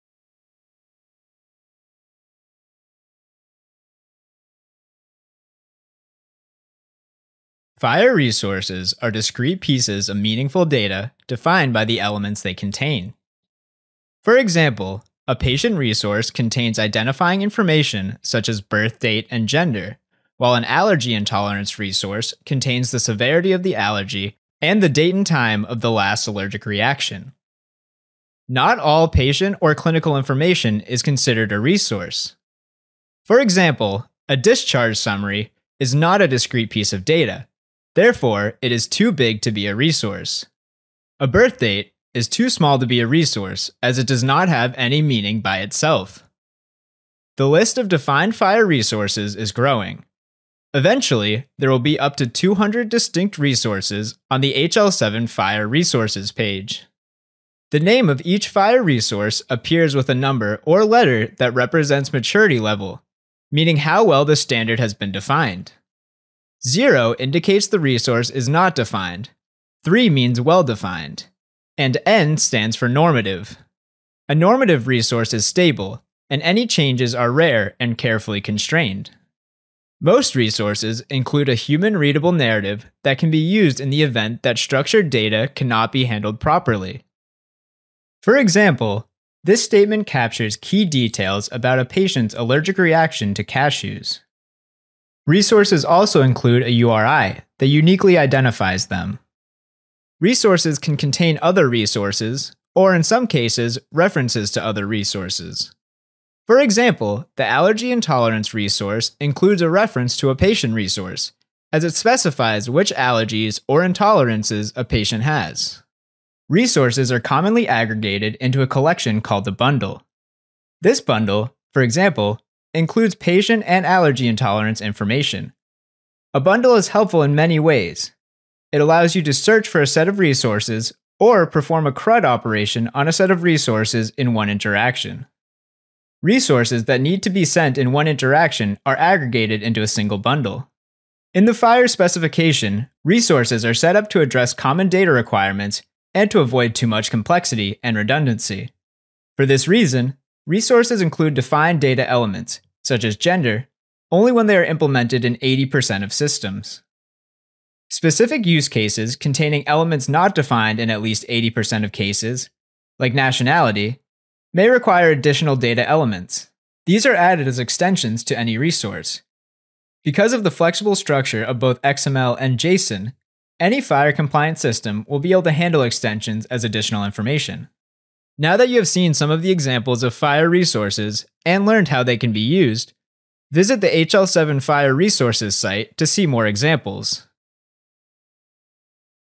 FHIR アーキテクチャ解説
新しい医療情報交換の標準である HL7 FHIRの仕組みについて、分かりやすく解説しています。日本語字幕付き。